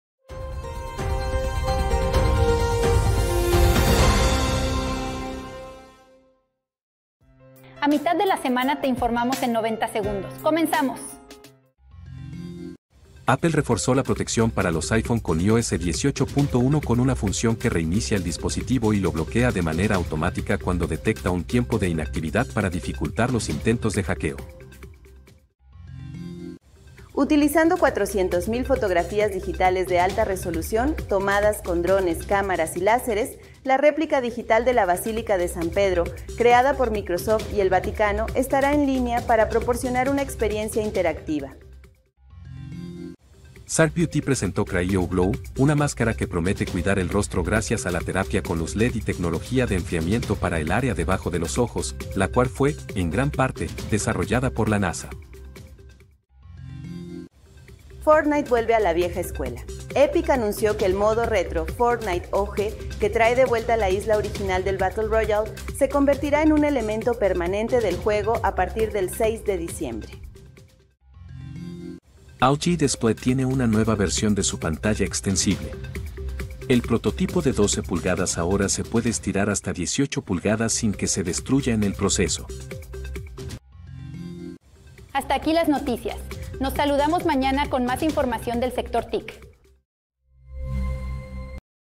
La Basílica de San Pedro a tu alcance
video | miércoles de cafeinfochannel : microsoft y elvaticano replican la basílica de San Pedro con más de 400 mil fotos digitales y crear una experiencia inmersiva; apple refuerza la protección de iphone 18 y evitar intentos de hackeo; y en gaming , forniteog trae de vuelta la isla original del juego en diciembre 2024.